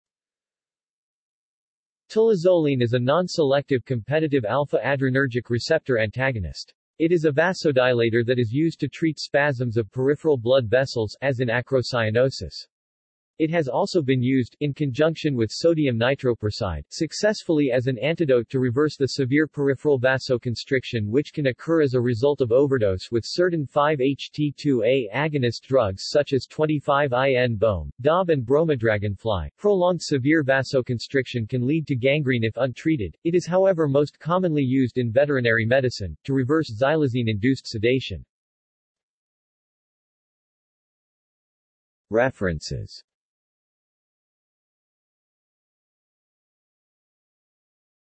Tolazoline | Wikipedia audio article
This is an audio version of the Wikipedia Article:
Tolazoline

SUMMARY
Tolazoline is a non-selective competitive α-adrenergic receptor antagonist. It is a vasodilator that is used to treat spasms of peripheral blood vessels (as in acrocyanosis). It has also been used (in conjunction with sodium nitroprusside) successfully as an antidote to reverse the severe peripheral vasoconstriction which can occur as a result of overdose with certain 5-HT2A agonist drugs such as 25I-NBOMe, DOB and Bromodragonfly (prolonged severe vasoconstriction can lead to gangrene if untreated).It is however most commonly used in veterinary medicine, to reverse xylazine-induced sedation.